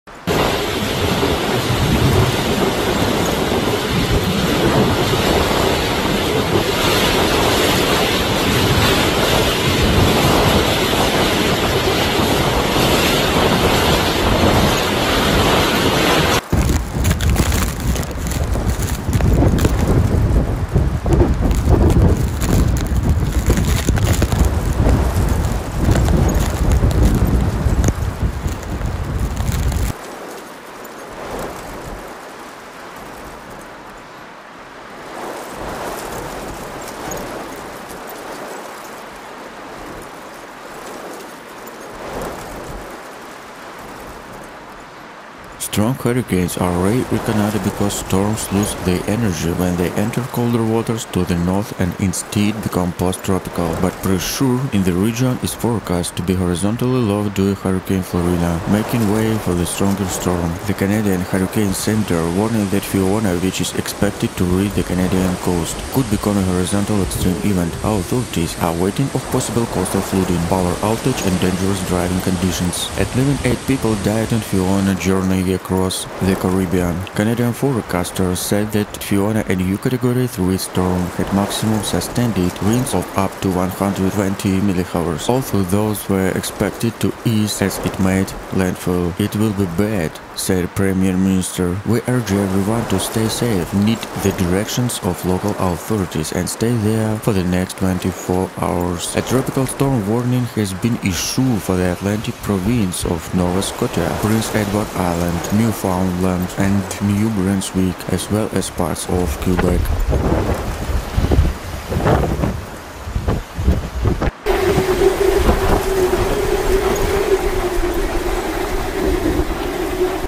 "Fiona" in Canada! The climate brings millions of losses to people! Footage from the event!
The Canadian Hurricane Center warned that Fiona, which is expected to reach the Canadian coast, could become a "historic extreme event".
Authorities are warning of possible coastal flooding, power outages and dangerous driving conditions.